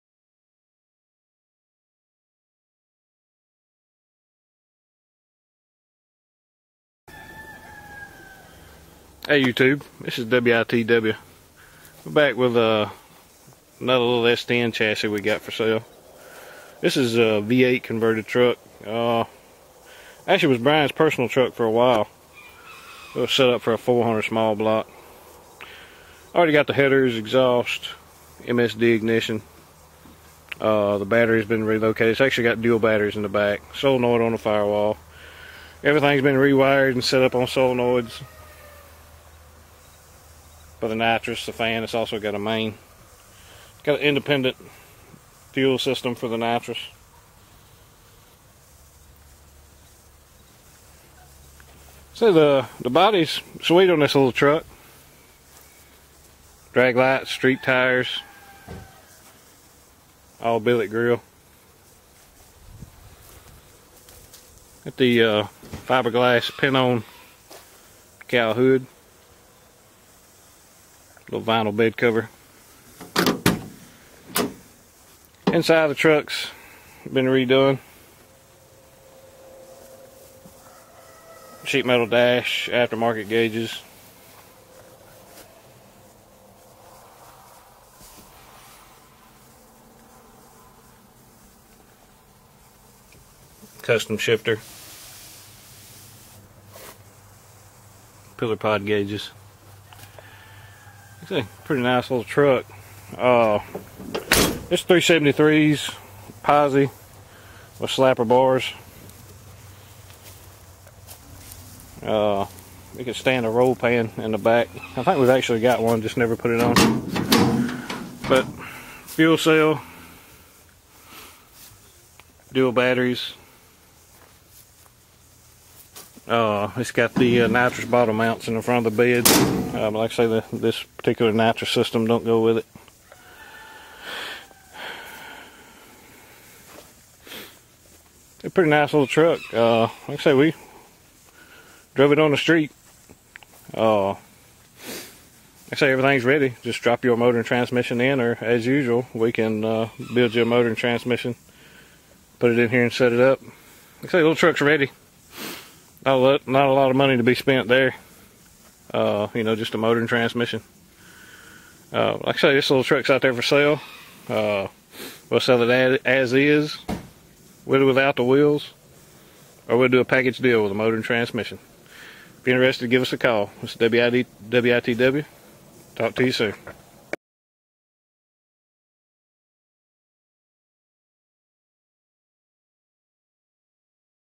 84 S10 For Sale
Sweet little 84 S10 street/strip chassis. Lots of cool extras! WITW Performance